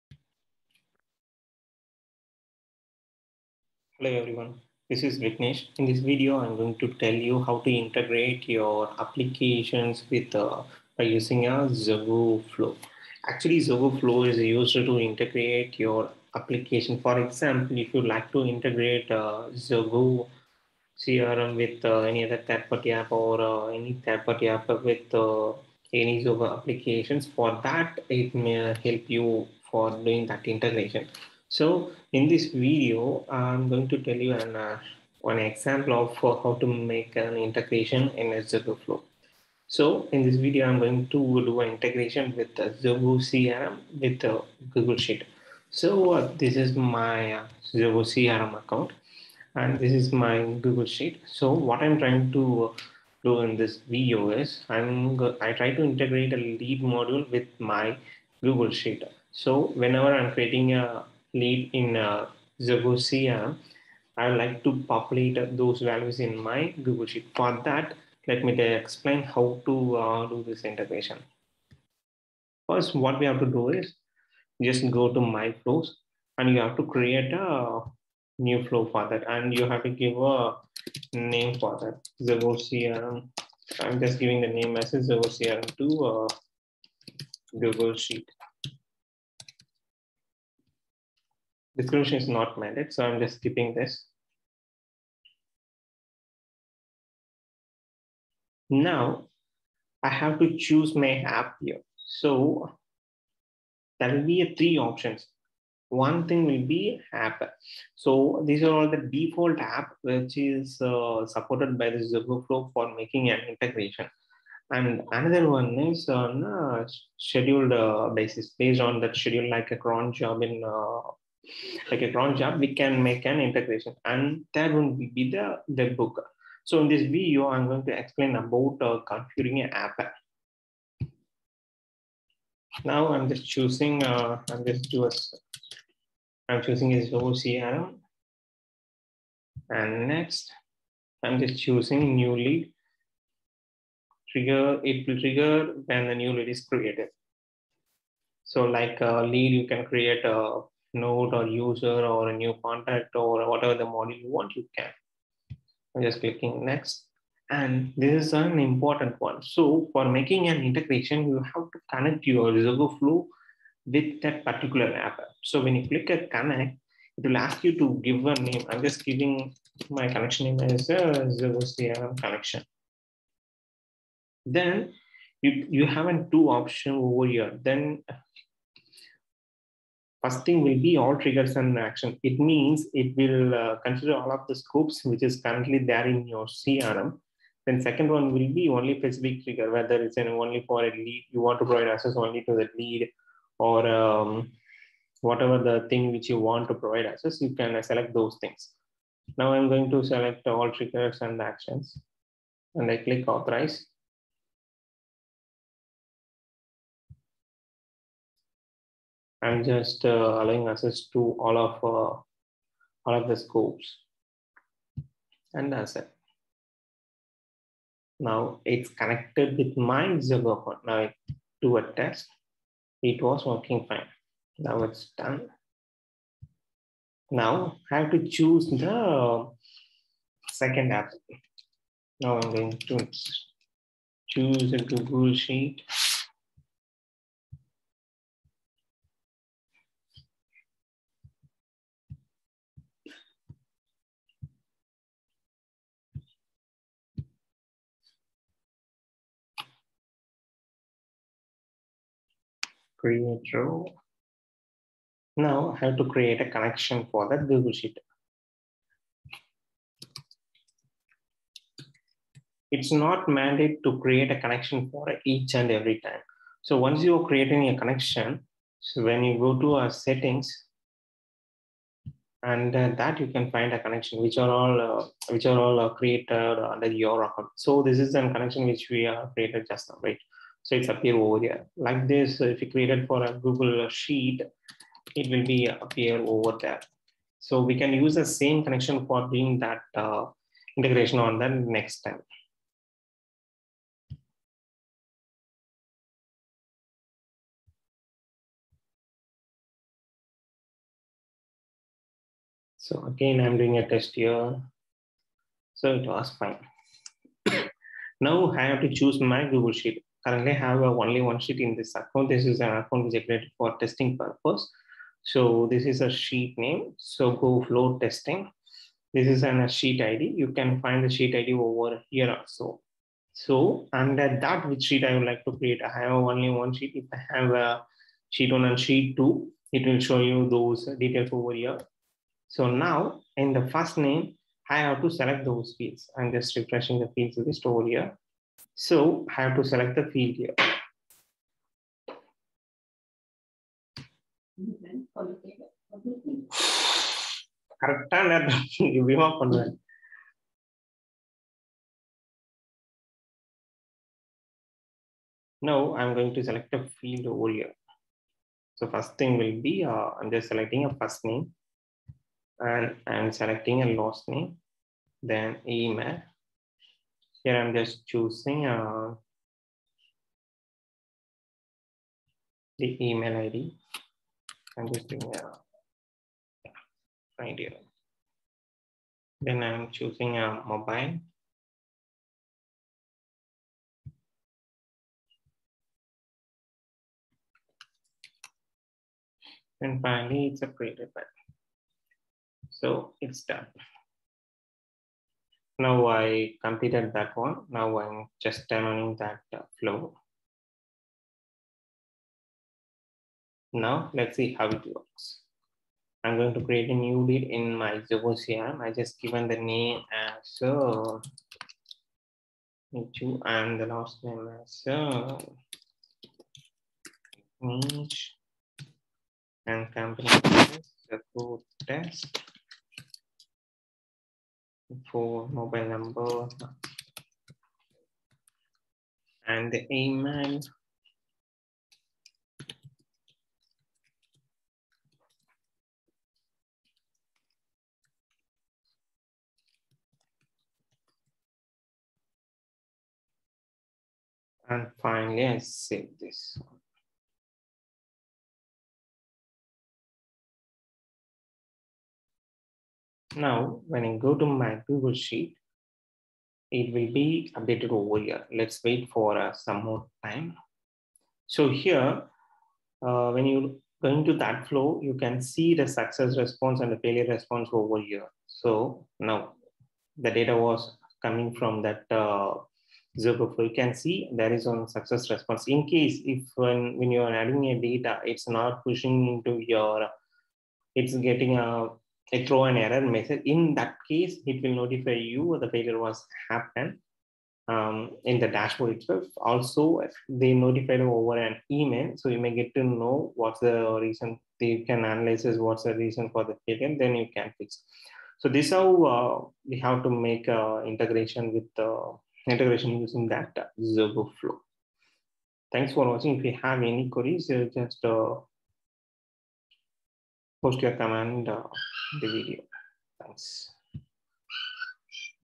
Zoho Flow Integration
This is a video about how to Integrate Apps using Zoho Flow
Zoho Certified Developer